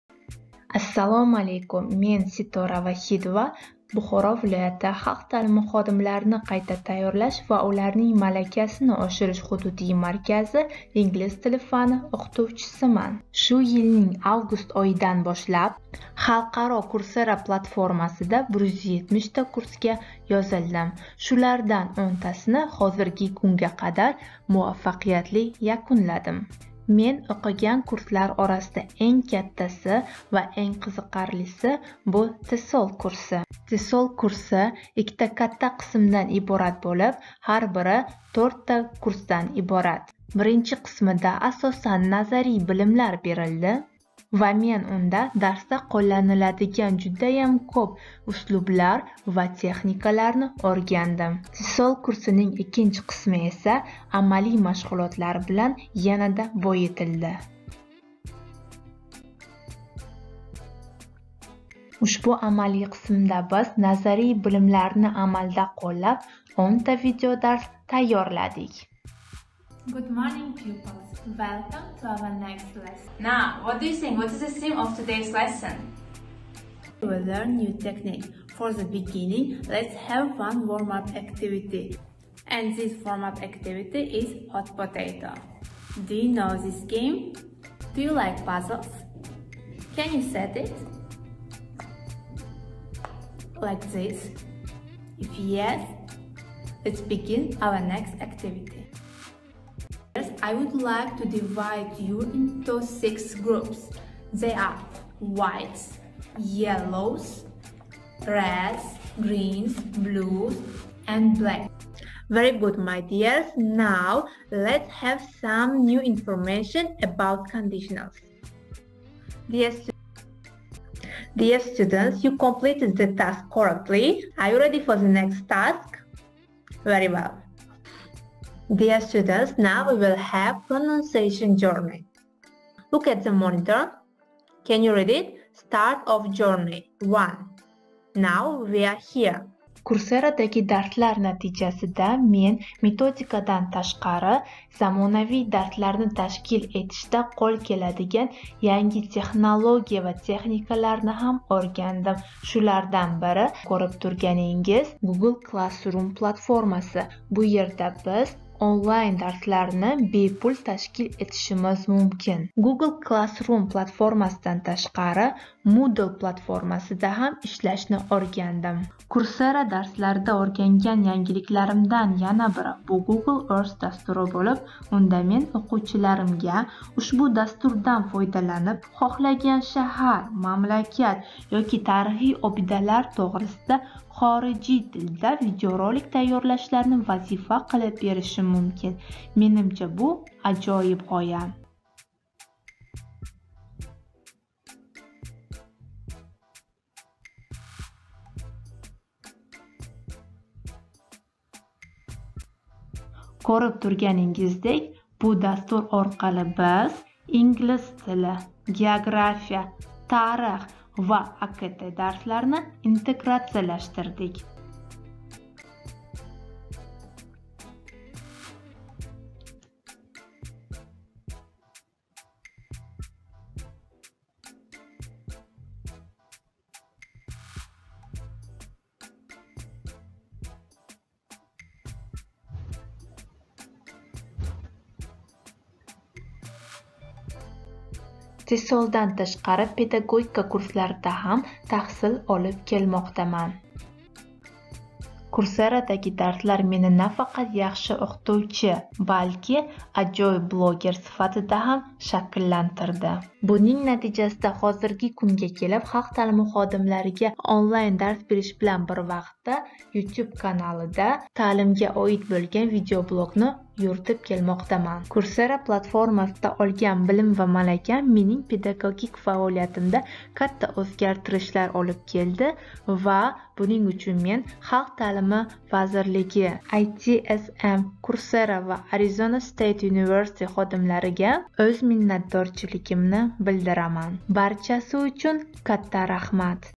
MENING COURSERA HAQIDA TAASSUROTLARIM / My impression about coursera /Мои впечатления о coursera.org
O'zbekiston Respublikasi Xalq Ta'limi Vazirligi va ITSM tanlovi uchun tayyorlangan video-loyiha.